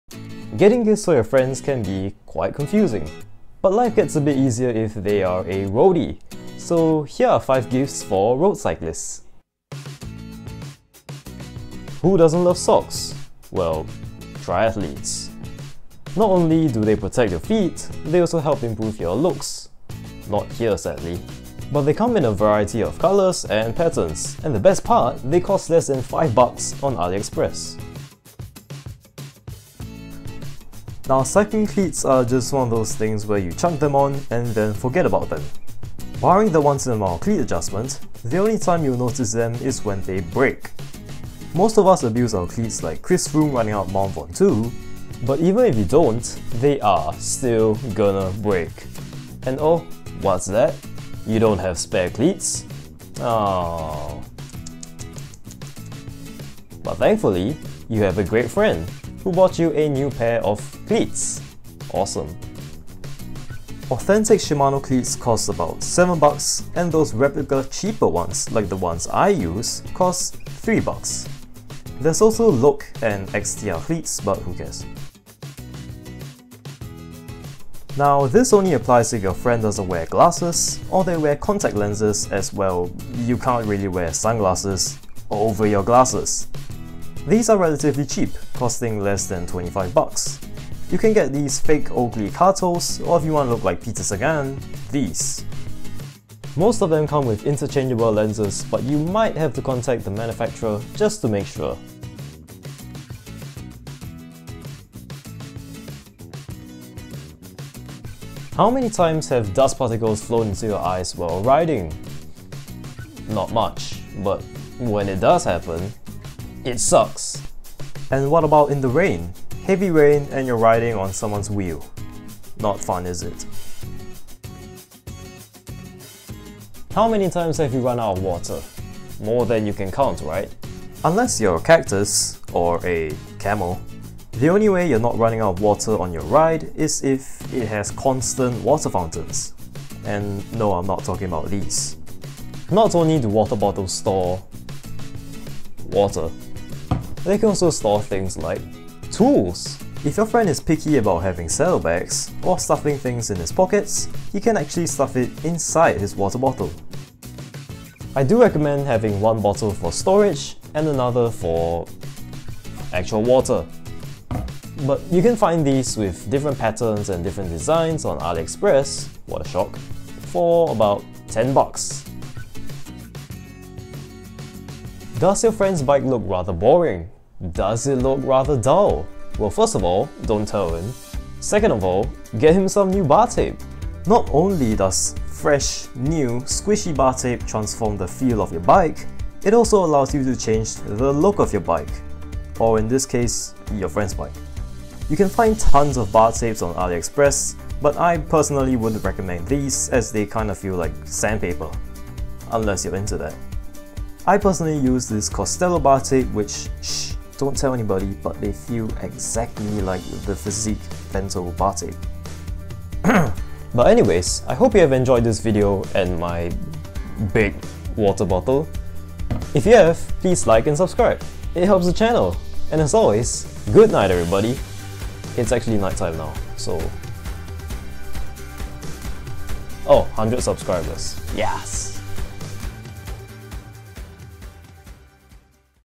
5 Super CHEAP Gifts for Road Cyclists
Birthdays, Christmas, or just for fun? Heres 5 super cheap gifts for Road Cyclists